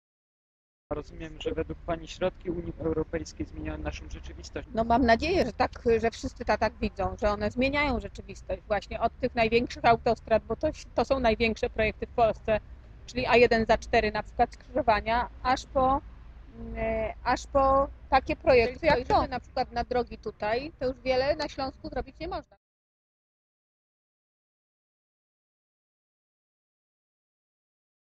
[fundeu.tychy.pl] Wywiad z Elżbietą Bieńkowską - zapowiedź #1
Zapowiedź wywiadu z MRR, panią Elżbietą Bieńkowską dla fundeu.tychy.pl - już we wtorek, 11 października!
Zapraszamy na fundeu.tychy.pl